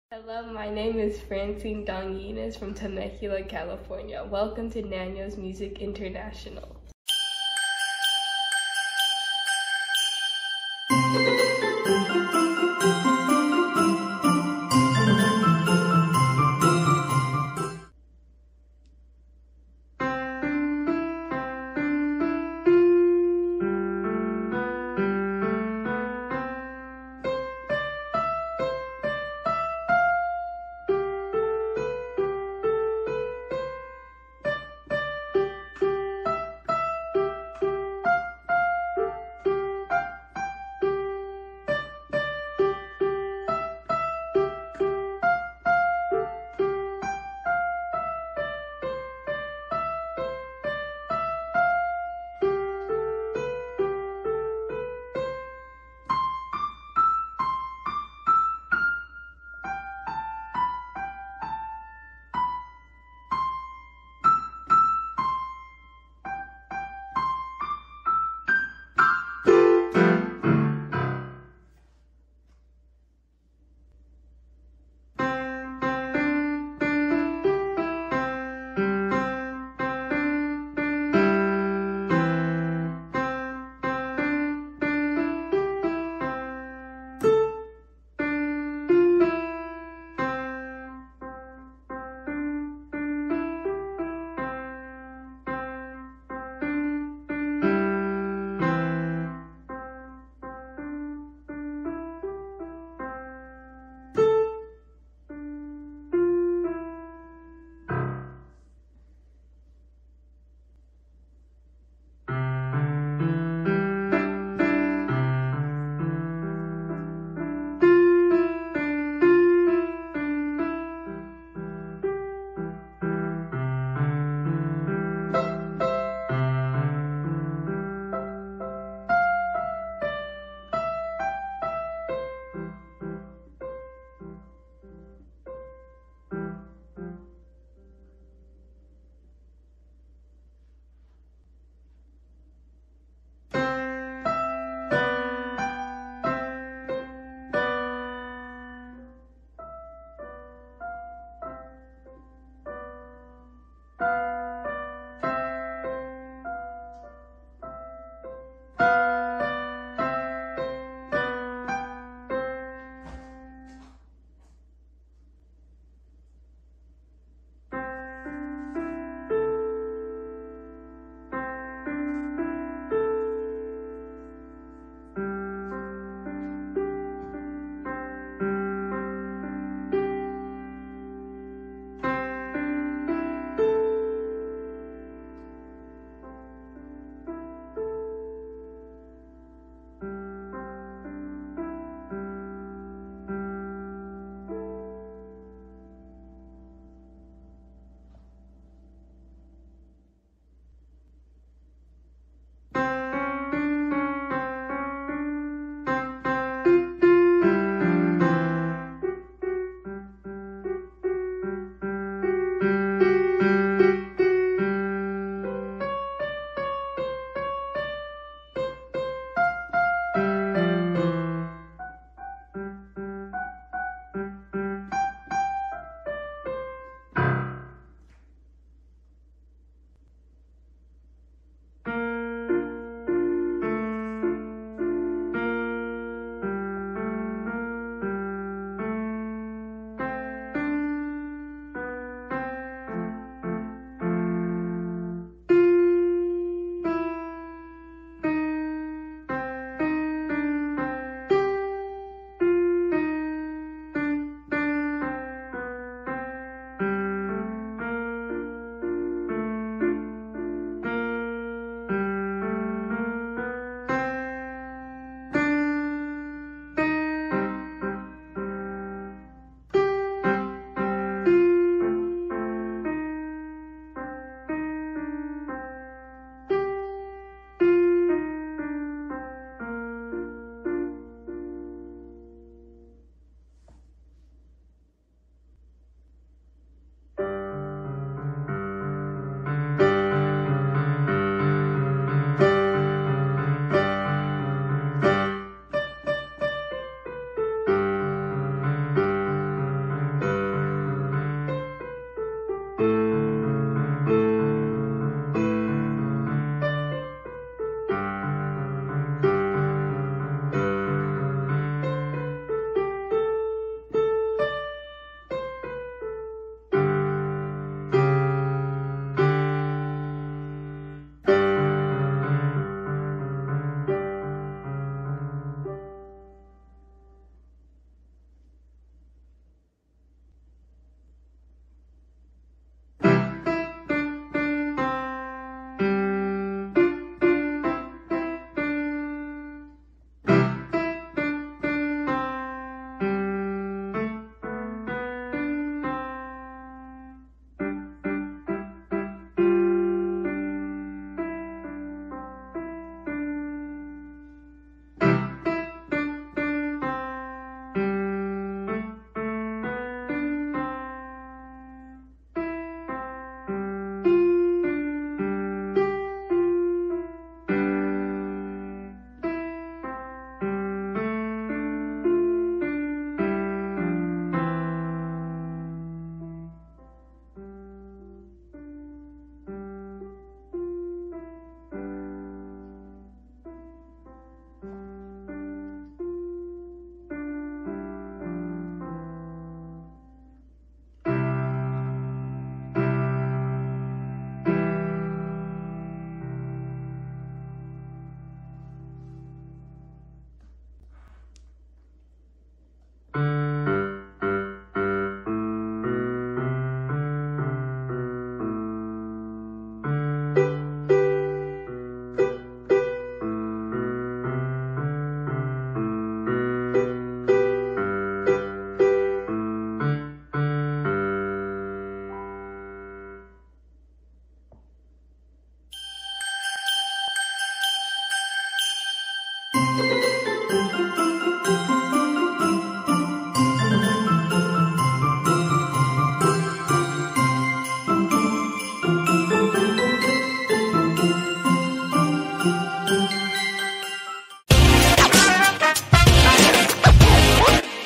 PIANO ADVENTURES - PERFORMANCE BOOK LEVEL 1 | FRANCINE
Here's our online piano student, Francine Donguines from San Diego, CA, playing ten (10) selected piano pieces from Piano Adventures Performance Book Level 1.